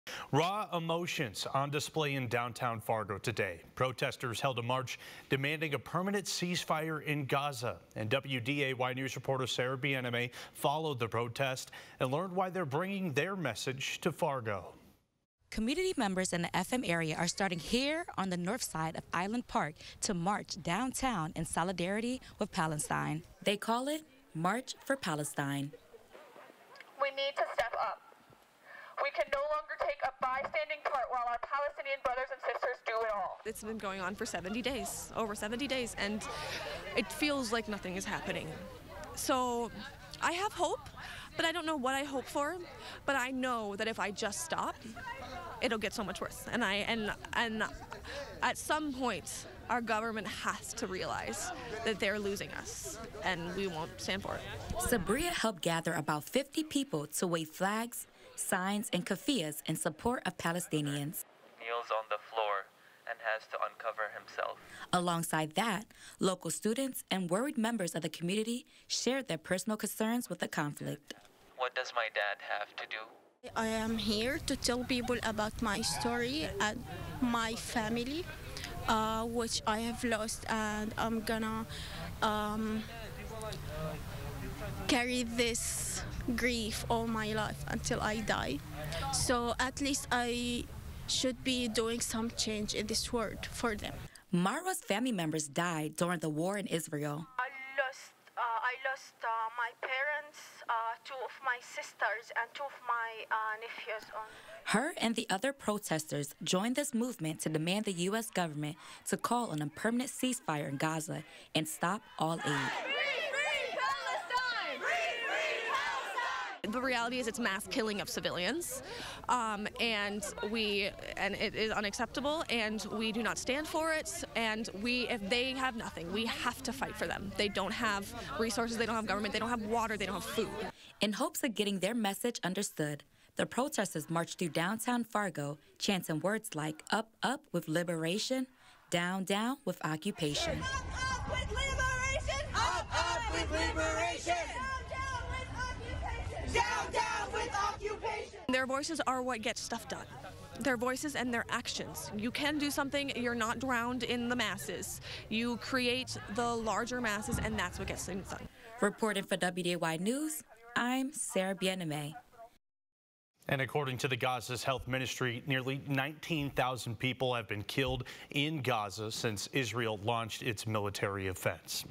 March for Palestine
Community members in the Fargo-Moorhead area marched again in solidarity for Palestine.